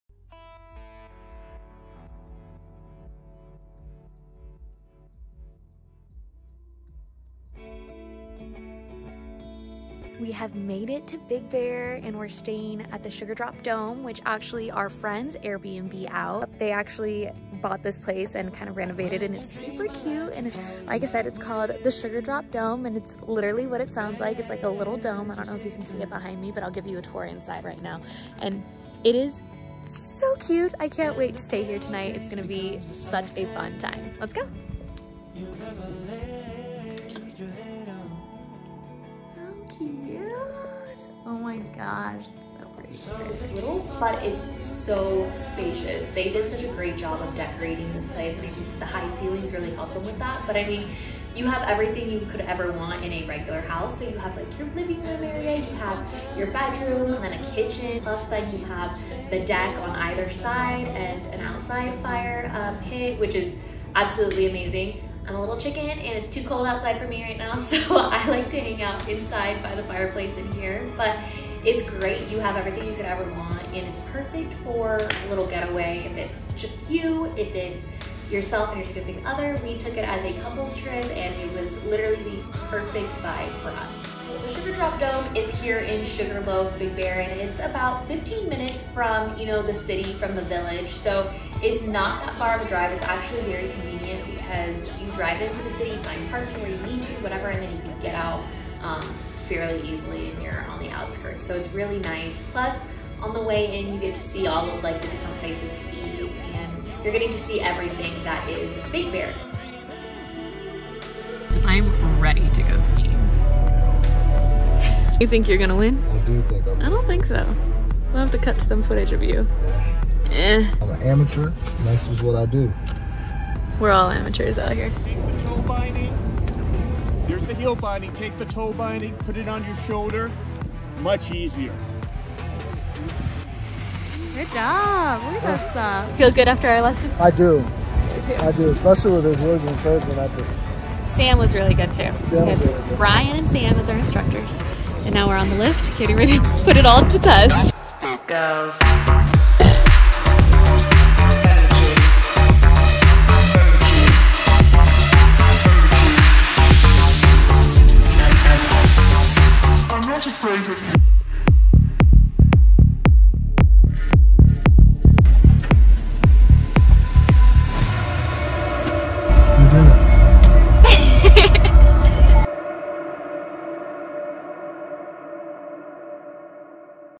Sweetest Stay-cation at the Sugar Drop Dome Airbnb
Stay-cation in Big Bear, CA, The Sugar Drop Dome was straight out of a fairytale, and the perfect way to unplug and disconnect from the world while relaxing and enjoying everything the dome has to offer, which was a lot!